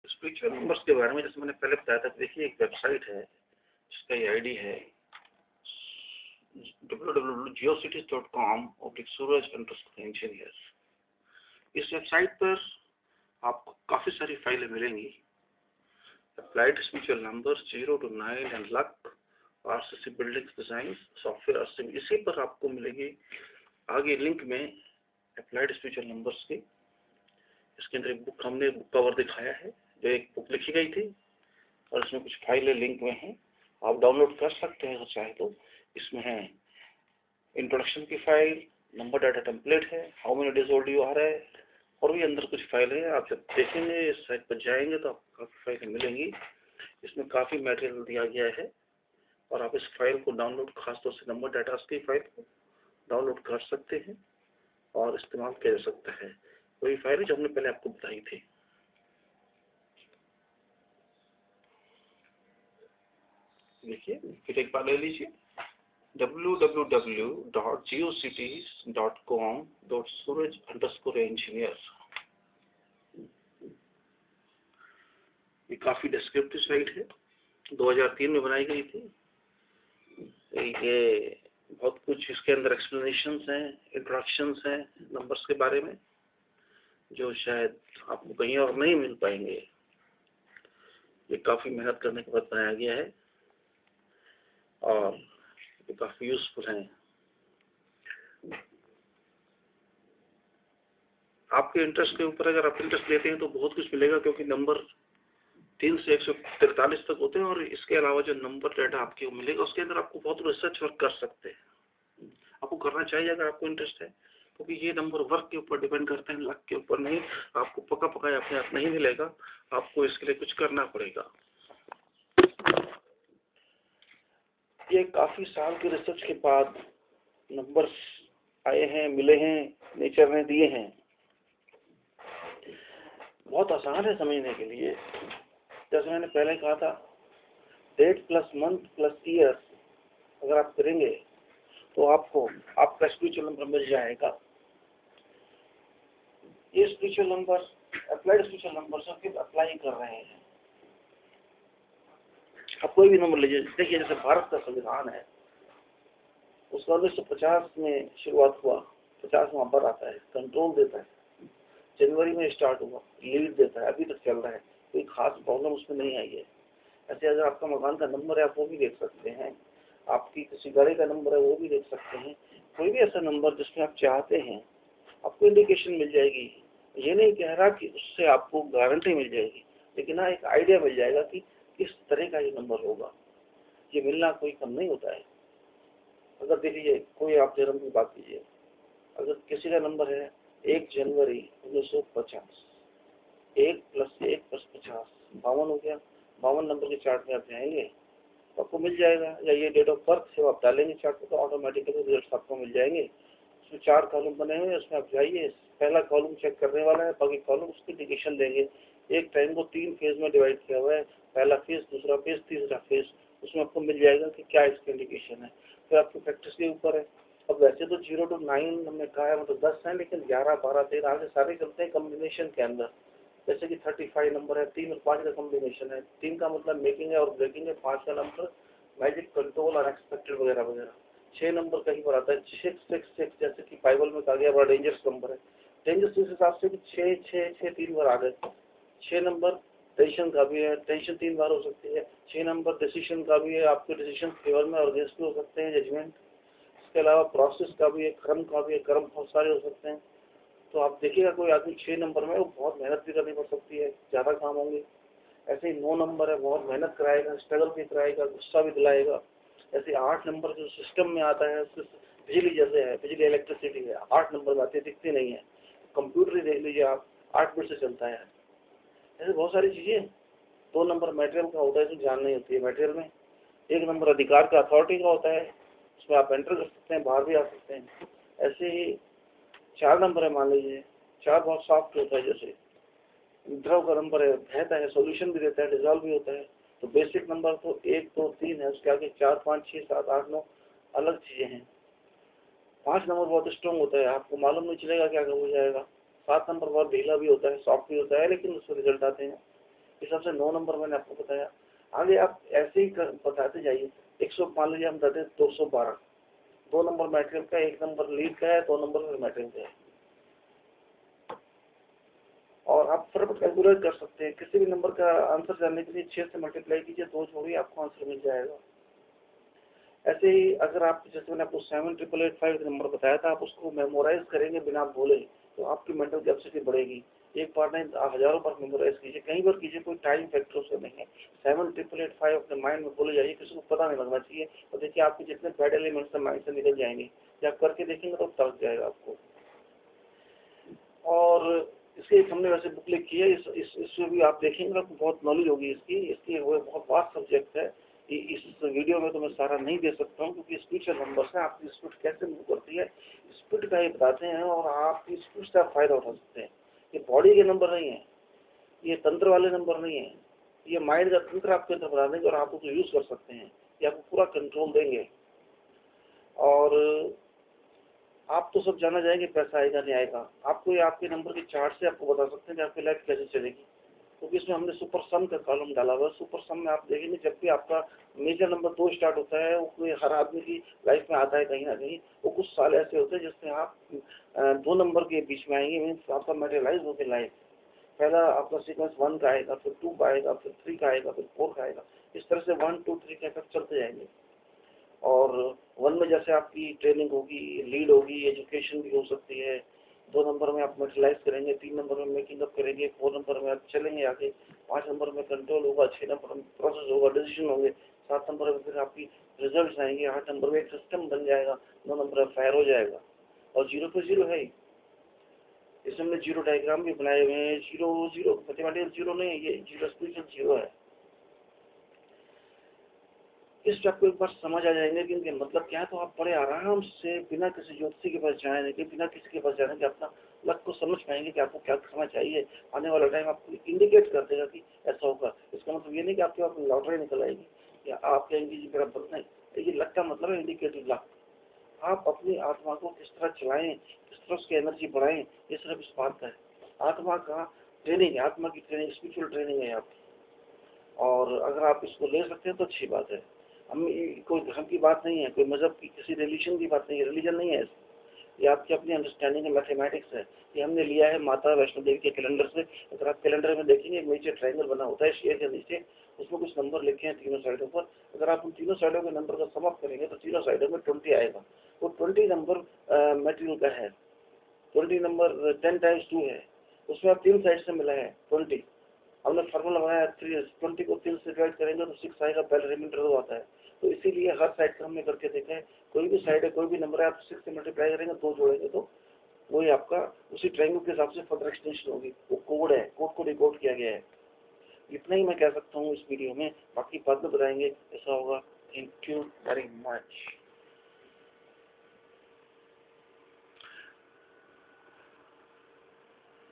Applied Spiritual Numbers 0 To 9 & Luck-Explanation Pertaining Numbers For Your Understanding
This video is third file giving further explanation about the Applied Spiritual Numbers referring to the website that can be visited for getting & downloading various informations from the url.
The file is in Exl  & word formats, easy to read & use
The whole subjec on the work is easy to understand
You shall find the work interested to make you happy.
P Eng Suraj Singh
Eng Suraj Singh PE

Engineer: Eng Suraj Singh